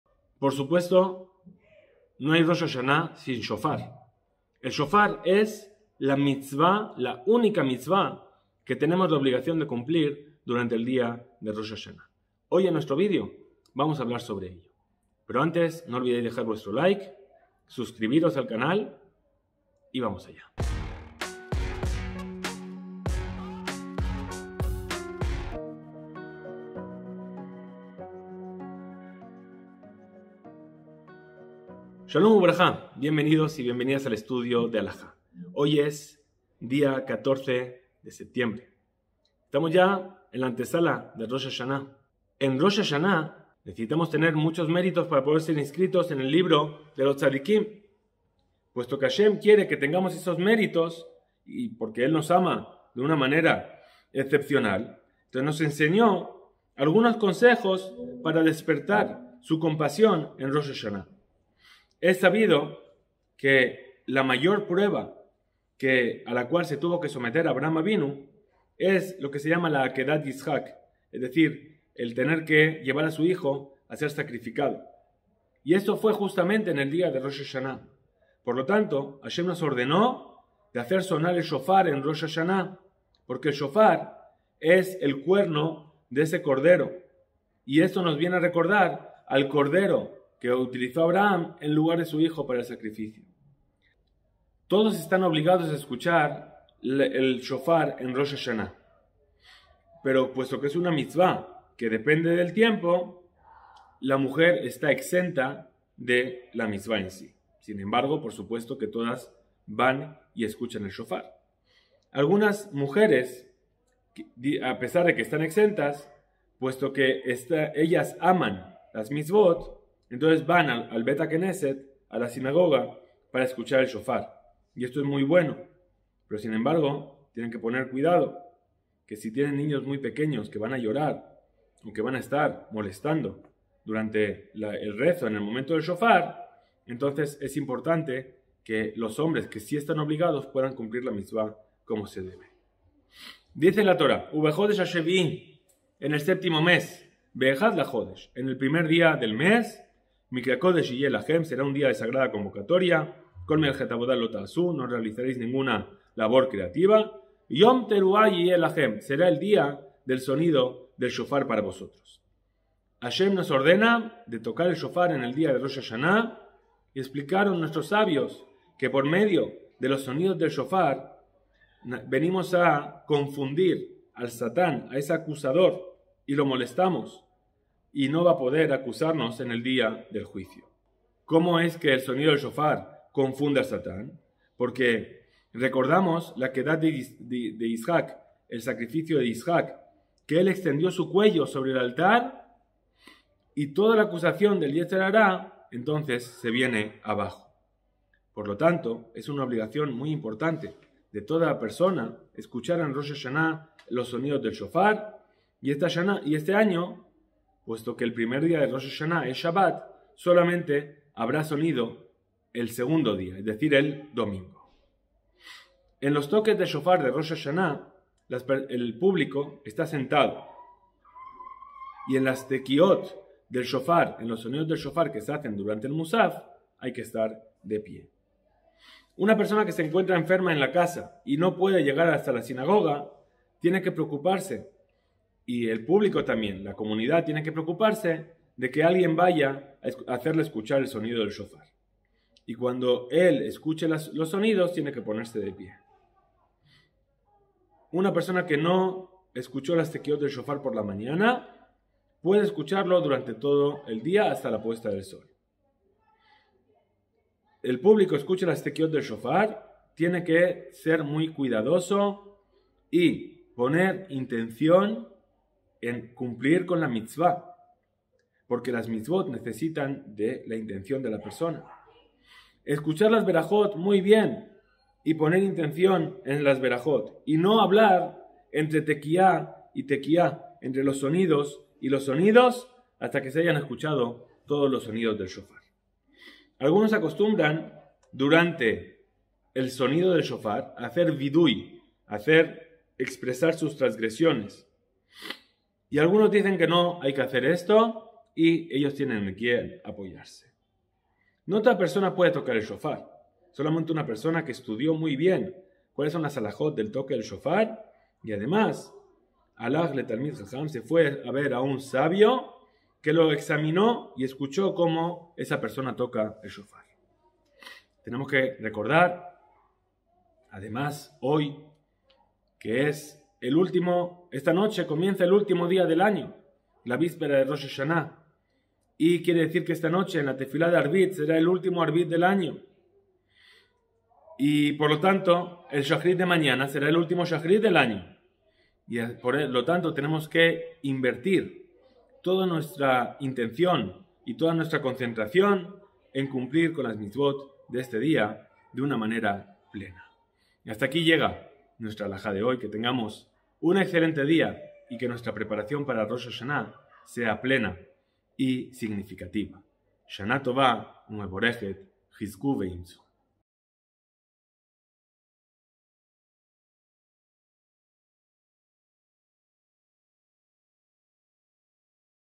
Halaja diaria - Preparandonos para Rosh HaShana - 14 de Septiembre
¡Bienvenidos a nuestro canal de YouTube! En este video, exploraremos las enriquecedoras y conmovedoras costumbres de la víspera de Rosh HaShaná, el Año Nuevo Judío. Desde la encantadora mesa festiva hasta la emotiva ceremonia de la shofar, te llevaremos en un viaje a través de las tradiciones que hacen que esta festividad sea tan especial. Acompáñanos mientras descubrimos el significado detrás de cada ritual y compartimos la esencia de la unidad familiar y la renovación espiritual que caracterizan a Rosh HaShaná. ¡No te pierdas esta inmersión en una de las festividades más importantes del calendario judío!